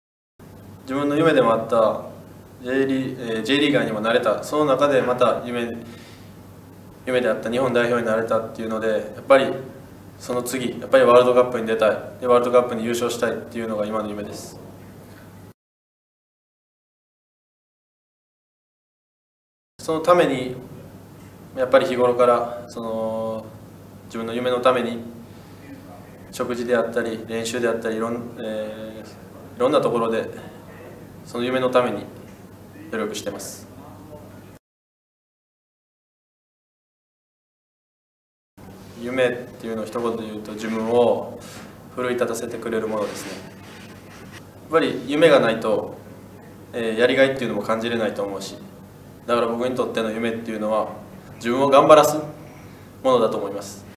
【サッカー日本代表 柿谷曜一朗】夢はワールドカップでの優勝すること！
柿谷曜一朗　　夢はワールドカップでの優勝すること！